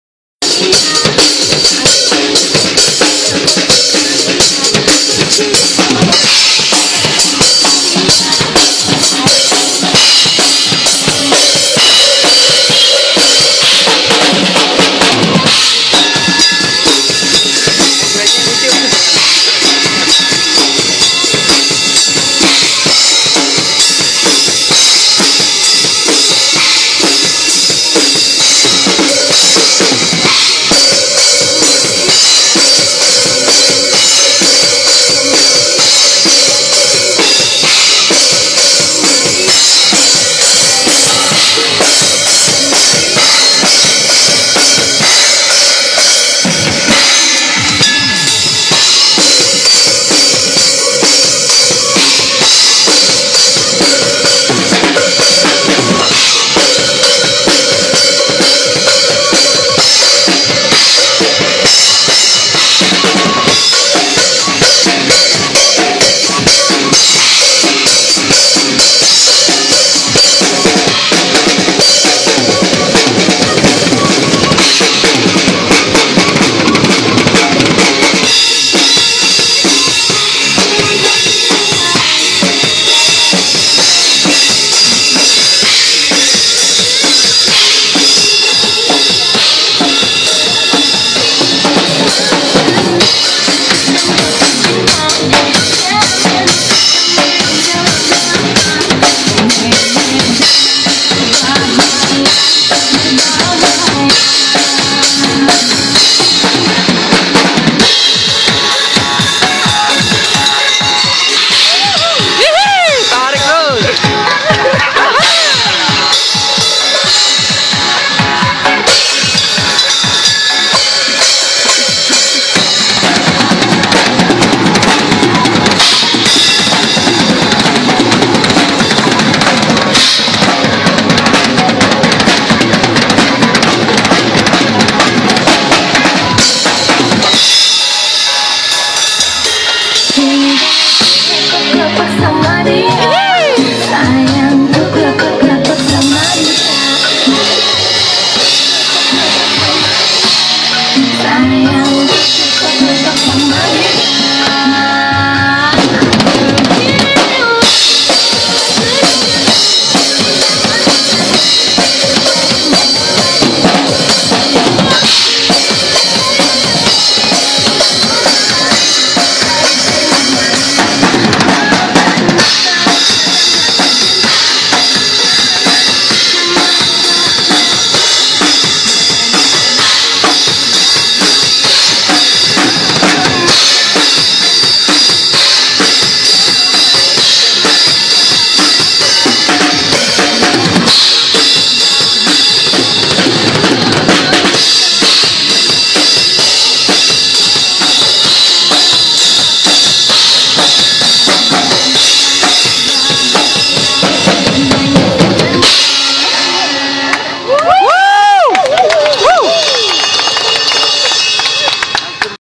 IDI SURABAYA GOES TO IDI NGANJUK by Helmy Newtron - Ikatan Drummer Indonesia
acara ini adalah acara kopdar / gathering IDI regional nganjuk yang sedang dikunjungi oleh IDI regional surabaya. drum dimainkan oleh Helmy Newtron, beliau adalah pemilik studio 68 nganjuk dan sekaligus Koordinator IDI regional nganjuk.